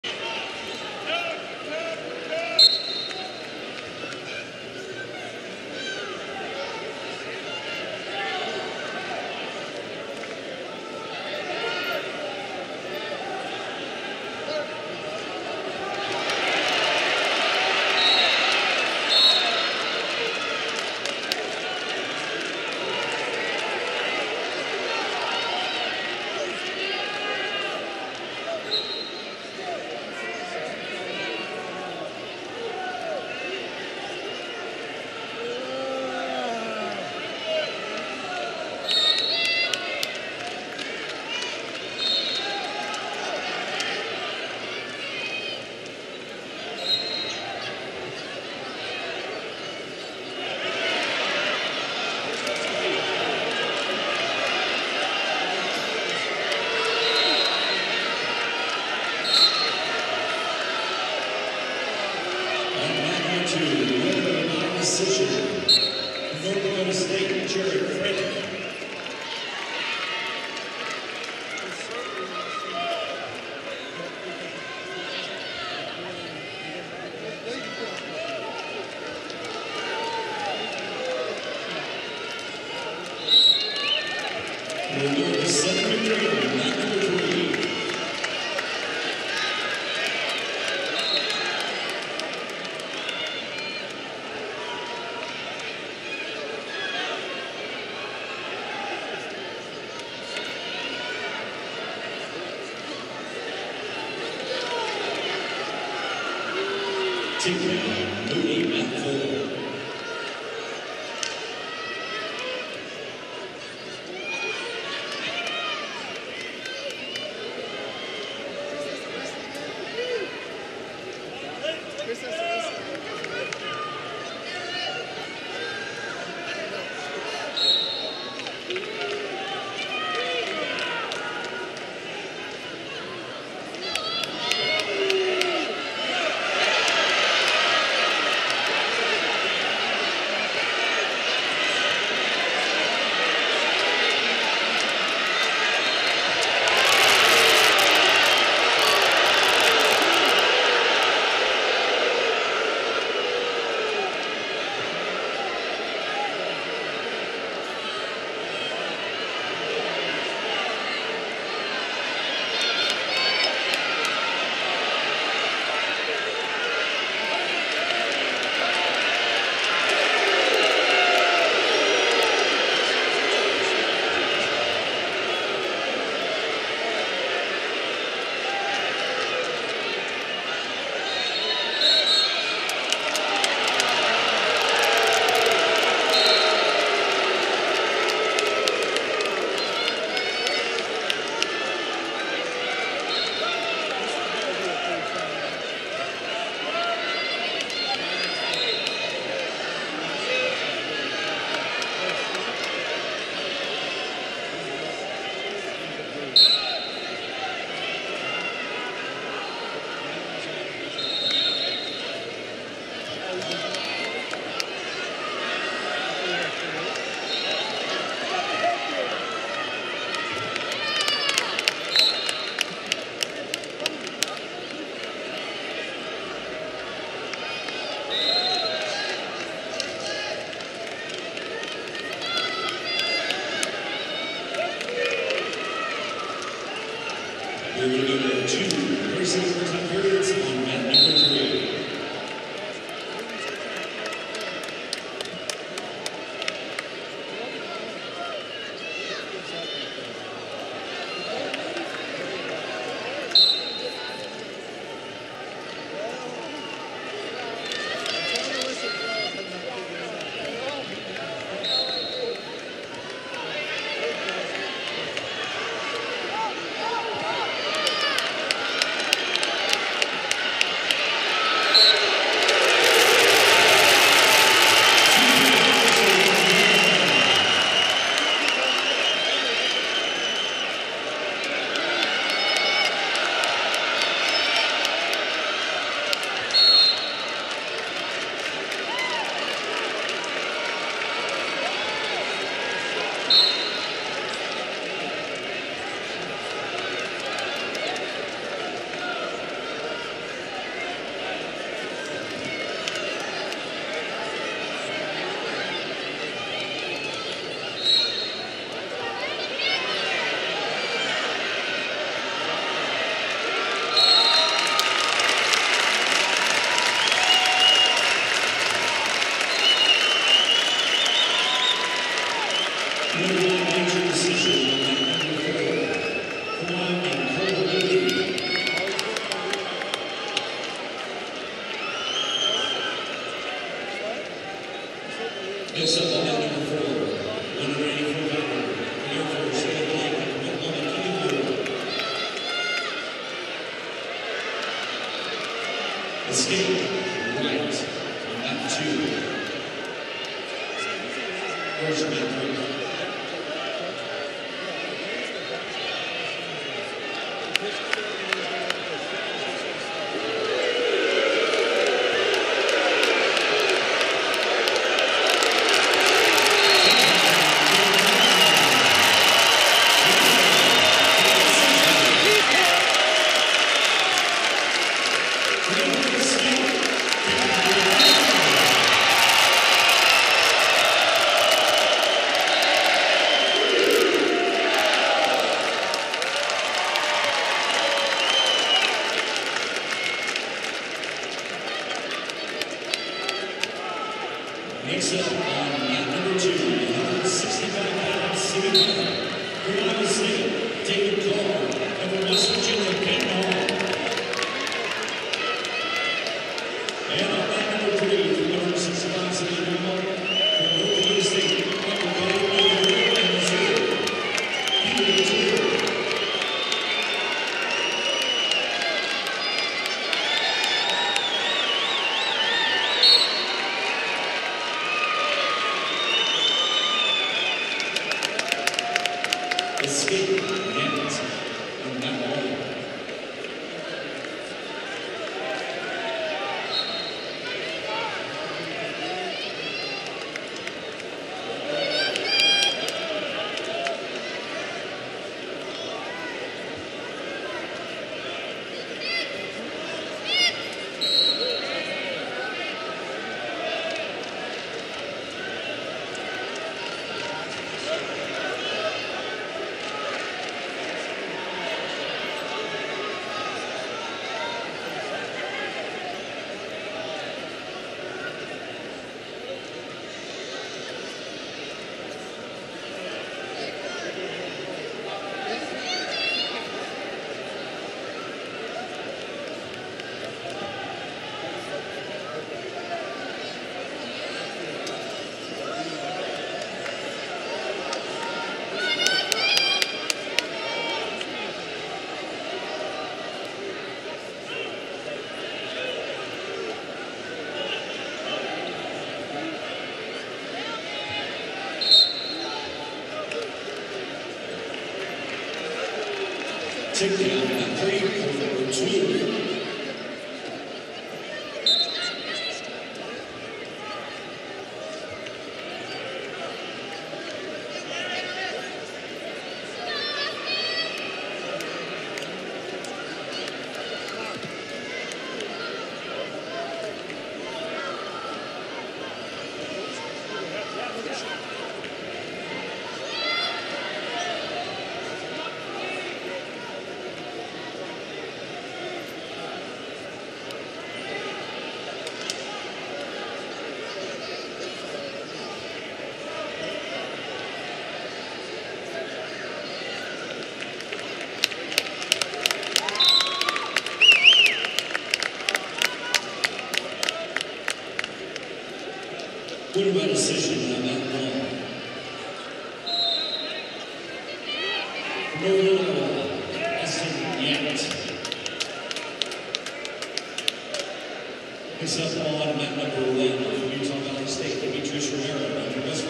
165lbs Austin Yant (Northern Iowa) vs Gerrit Nijenhuis (Oklahoma)
2023 Big 12 Wrestling Championships
consolations 165lbs Austin Yant (UNI) dec Gerrit Nijenhuis (OU)